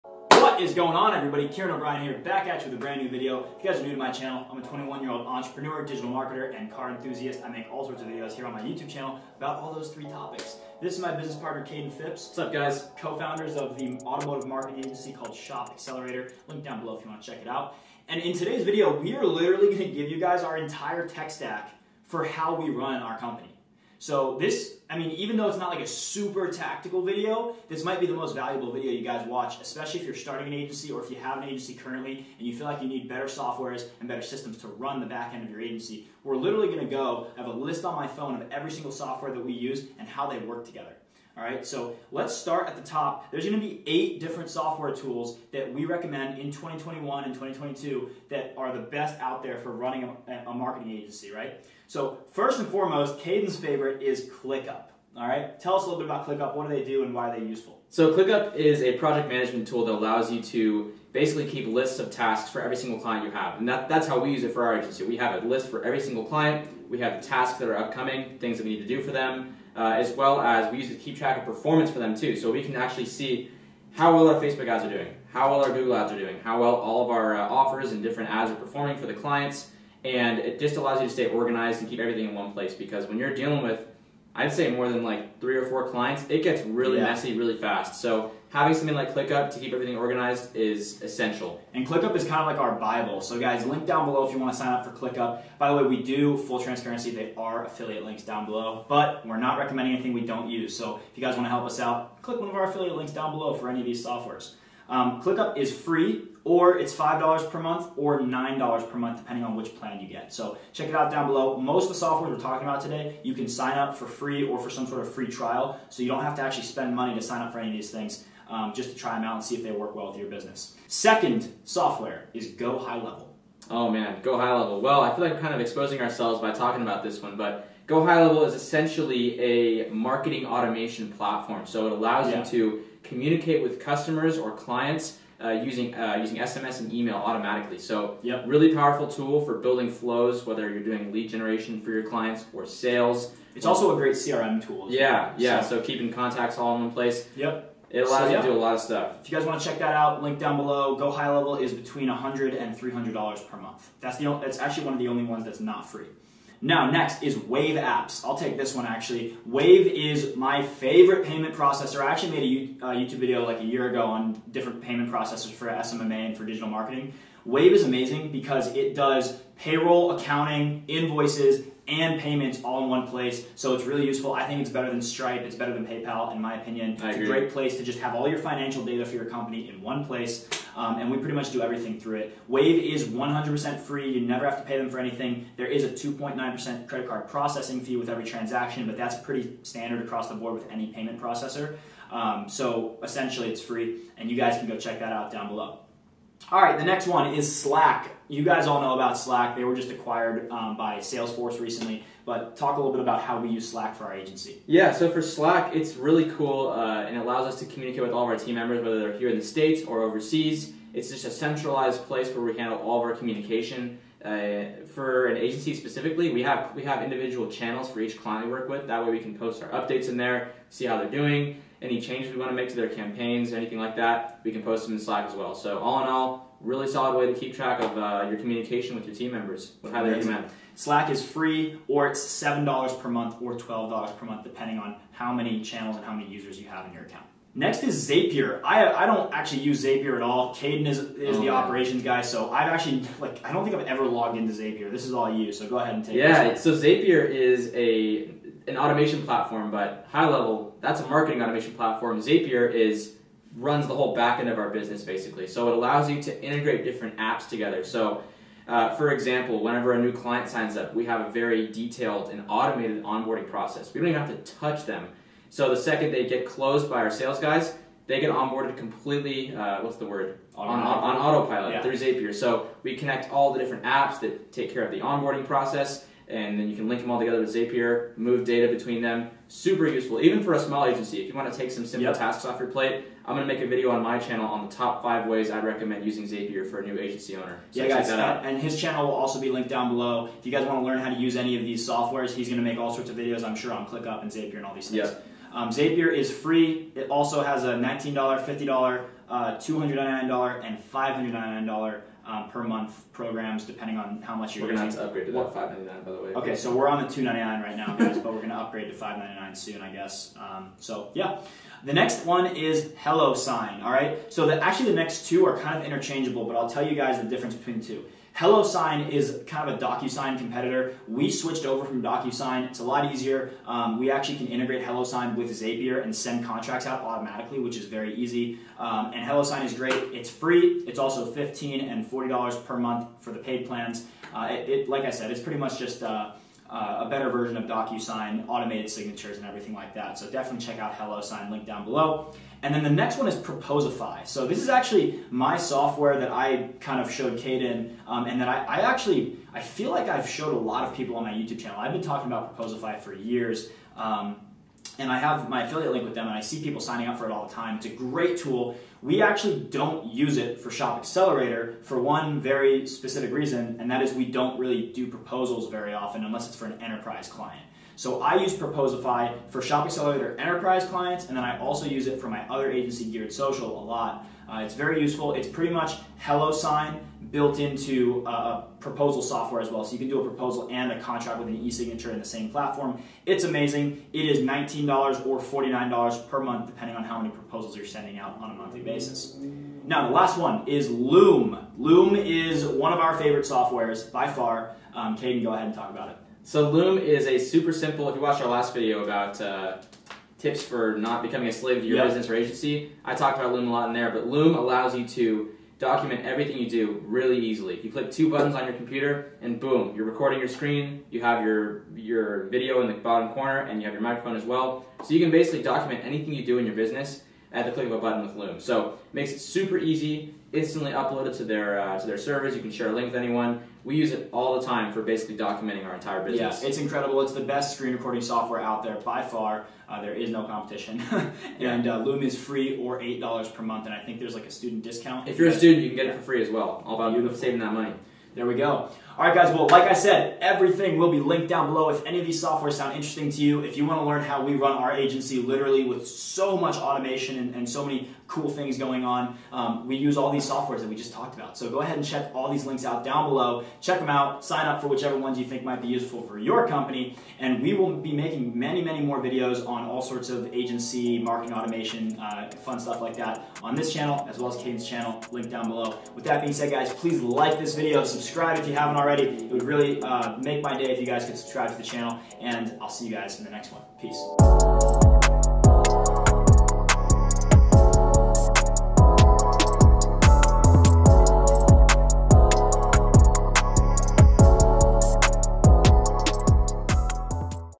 Top 8 BEST Tools & Software for Your Marketing Agency
All of the softwares and tools you NEED in 2021 to run and scale a marketing agency